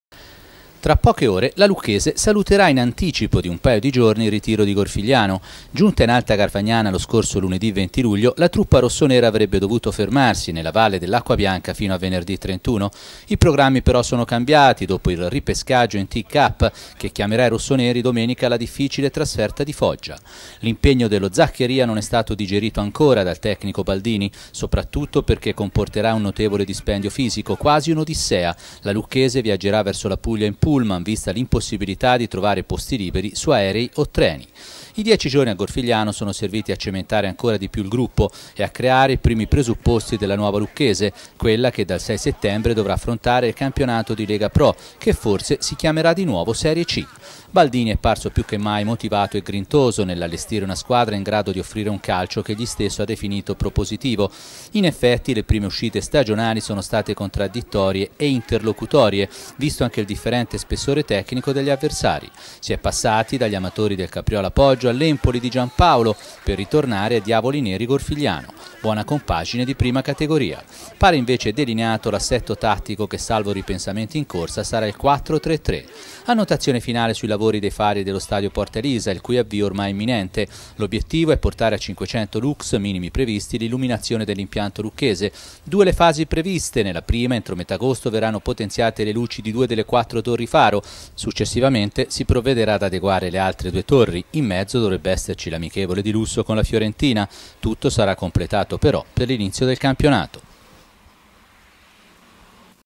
Lucchese saluta il riiro di Gorfigliano
CALCIO LEGA PRO - Mercoledì pomeriggio si conclude il ritiro della Lucchese a Gorfigliano. I rossoneri chiudono in anticipo il ritiro in Alta Garfagnana per l'impegno di Foggia in Tim Cup. Al via i lavori all'impianto di illuminazione del Porta Elisa.

NoiTv